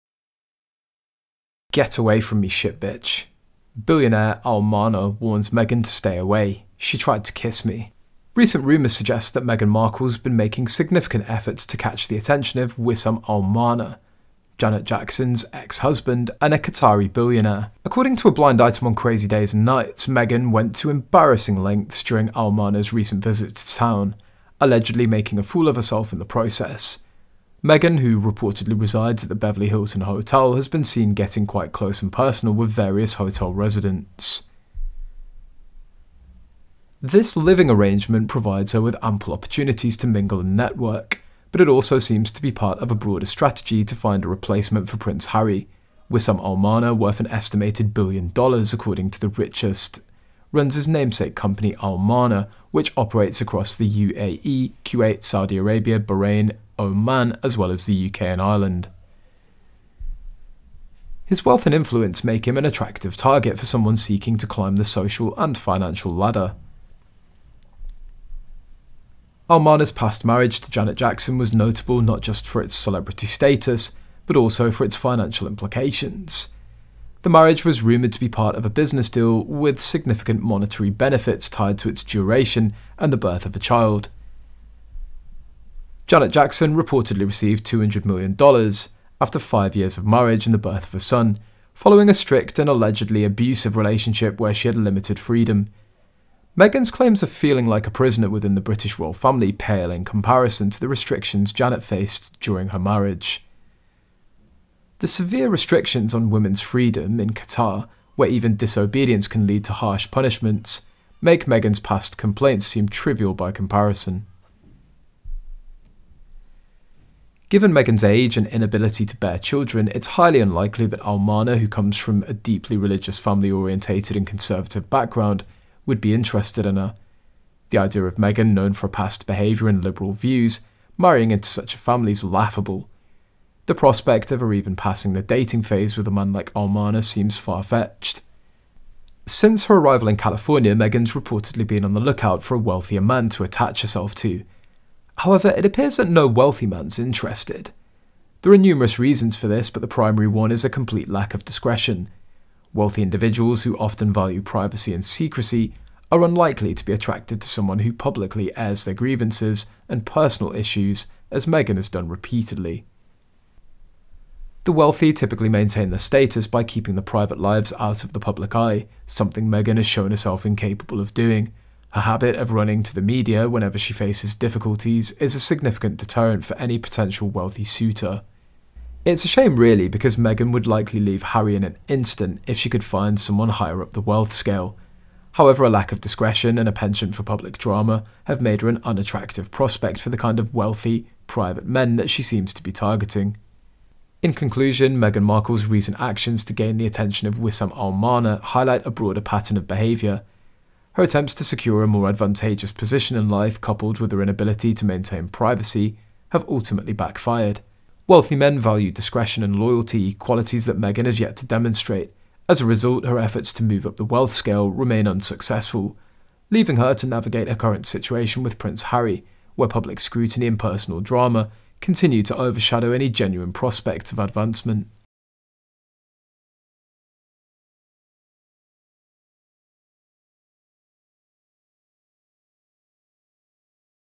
DISGUSTING! Billionaire Al Mana Warns Meghan To Stay Away After She Try To Lure Him For Fame
Recent rumors suggest that Meghan Markle has been making significant efforts to catch the attention of Wissam Al Mana, Janet Jackson's ex-husband and a Qatari billionaire. According to a blind item on Crazy Days and Nights, Meghan went to embarrassing lengths during Al Mana's recent visit to town, allegedly making a fool of herself in the process. Meghan, who reportedly resides at the Beverly Hilton hotel, has been seen getting quite close and personal with various hotel residents.
This living arrangement provides her with ample opportunities to mingle and network, but it also seems to be part of a broader strategy to find a replacement for Prince Harry. Wissam Al Mana, worth an estimated billion dollars according to The Richest, runs his namesake company Al Mana, which operates across the UAE, Kuwait, Saudi Arabia, Bahrain, Oman, as well as the UK and Ireland. His wealth and influence make him an attractive target for someone seeking to climb the social and financial ladder.